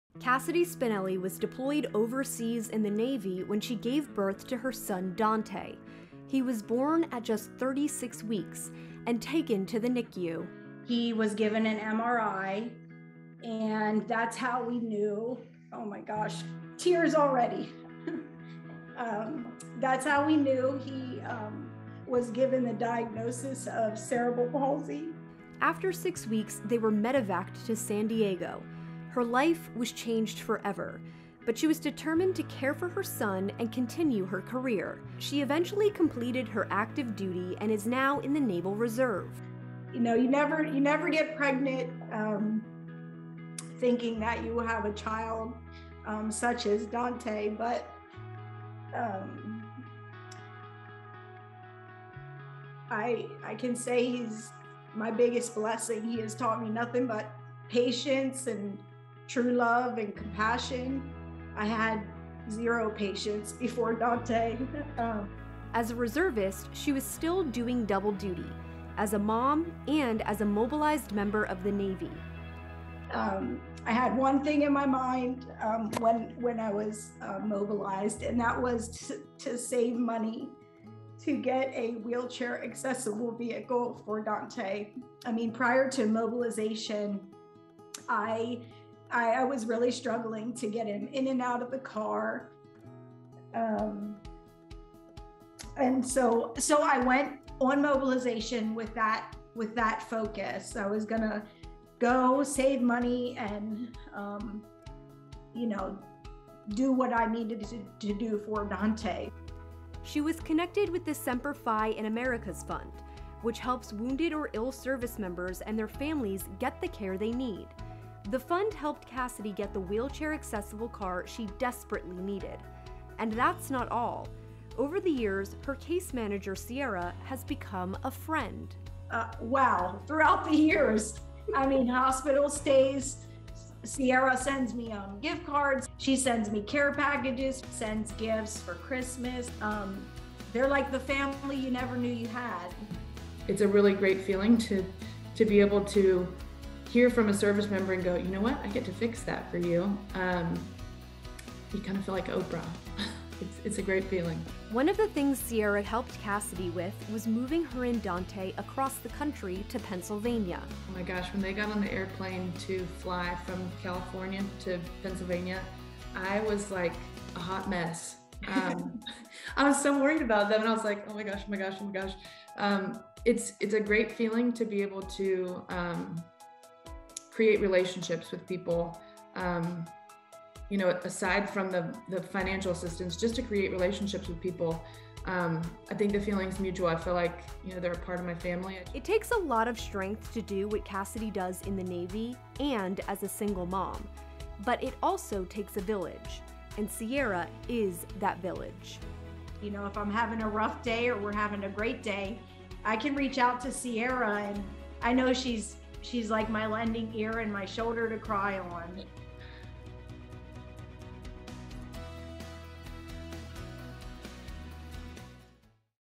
Military mom gets wheelchair accessible car for son – and so much more
Cassidy Spinelli was determined to balance her career in the Navy and her role as a mom. Her son, Dante, has cerebral palsy and she was desperate for a wheelchair-accessible car for him. That's when she found a group who helped her get one – and gave her so much more, including friendship.